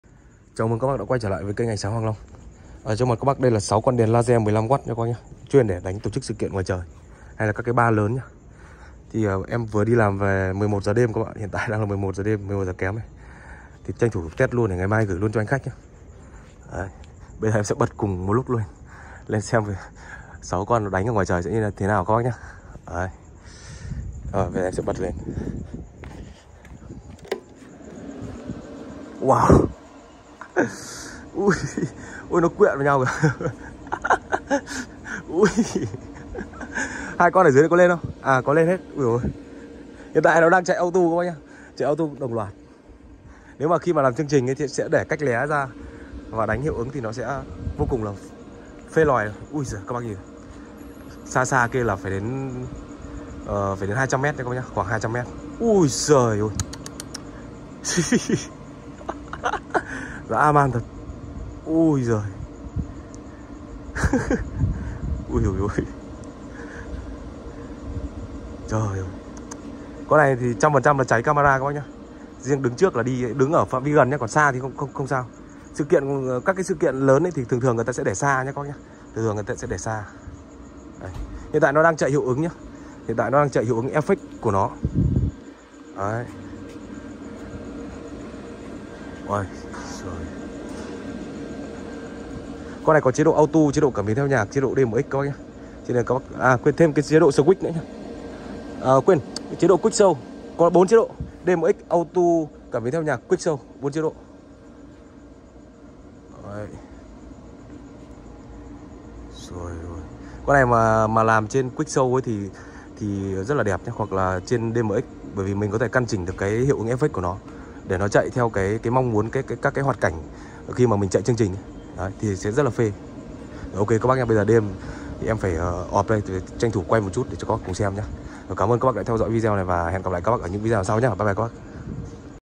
Tét Nhanh 6 Đèn Laser 15w
Thông Số Kỹ Thuật Laser M15
Công suất đèn laser : đỏ R 4000mw (638nm) - xanh lá G 4000mw (520nm) - xanh dương B 4000mw (450nm)
Tốc độ điện kế: điện kế 30K
Hiệu ứng chương trình: các chùm tia mầu đa sắc khác nhau, hình hình học, hình động, có hơn 300 hiệu ứng trong bộ nhớ trong đèn laser.
Hiệu ứng chức năng: Cảm biến theo nhạc, chạy auto, DMX512, QuickShow
Ứng Dụng : KTV, vũ trường, quán bar, biểu diễn nghệ thuật trong nhà, ngoài trời, đám cưới, phòng trà,vv...
Chế độ bảo hành : bằng team bảo hành
Thời gian bảo hành : 12 tháng
Kích thước đóng đèn trong thùng catton : 47cm x 37cm x 40.4cm
Kích Thươc Đèn : 25cm x 24cm x 30.5cm.
Cân Nặng Đèn Bao Gồm Catton : 11kg
Cân Nặng Đèn : 9kg